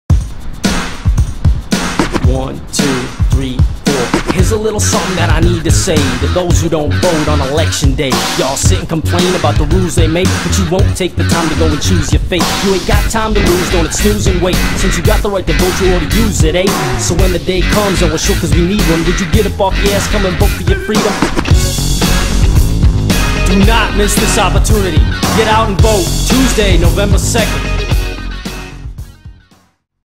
Right 2 Vote PSA
Public Service Announcement written & performed by KNIFF